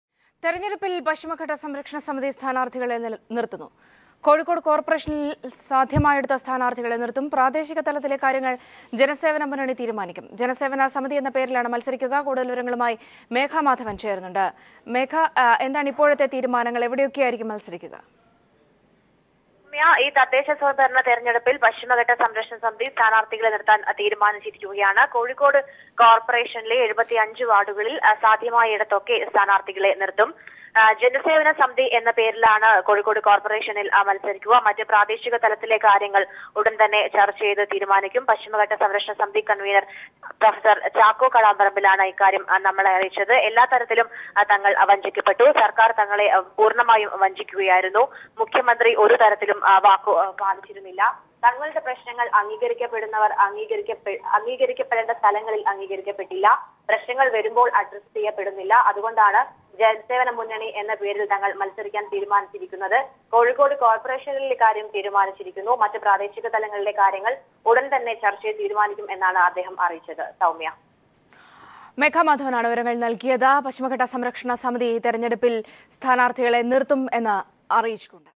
Panchayat Elections 2015: Paschima Ghatta Samrakshana Samithi to Field Candidates
The Paschima Ghatta Jana Samrakshana Samithi will field its candidates for the mandate of Kozhikode Corporation.